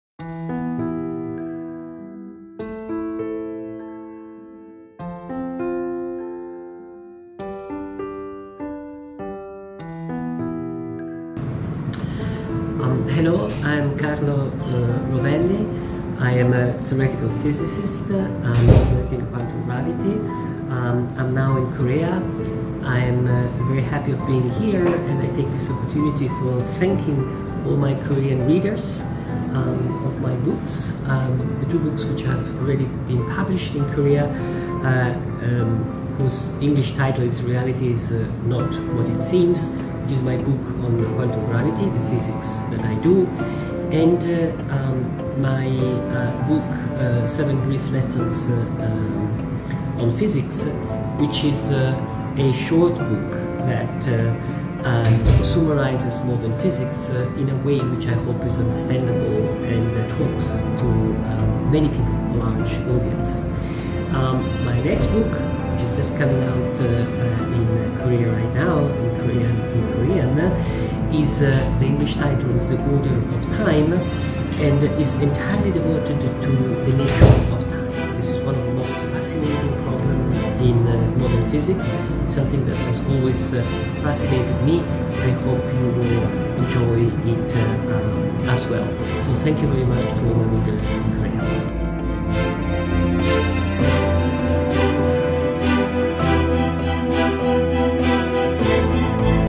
★세계적인 이론 물리학자 '카를로 로벨리' 전격 인터뷰!★
양자중력 이론의 선구자이자 세계적인 물리학자 카를로 로벨리의 세 번째 책 《시간은 흐르지 않는다》가 출간됐습니다.

《모든 순간의 물리학》, 《보이는 세상은 실재가 아니다》에 이은 이번 책은 양자중력 이론의 관점에서 바라본 '시간'에 관한 이야기. '시간이란 도대체 무엇일까?', '시간이 흐른다는 것은 실제로 어떤 의미일까?', '우리가 이곳에서 경험하는 시간과 우주의 시간은 다른 것일까' 이러한 질문들에 대한 카를로 로벨리의 충실한 답변서입니다.

✔ 2018 뉴욕 타임스 올해의 논픽션 BEST 10!
✔ "스티븐 호킹의 《시간의 역사》 이후,
물리학과 철학이 이렇게 아름답게 융화된 적은 없었다."
_'옵저버(The Observer)'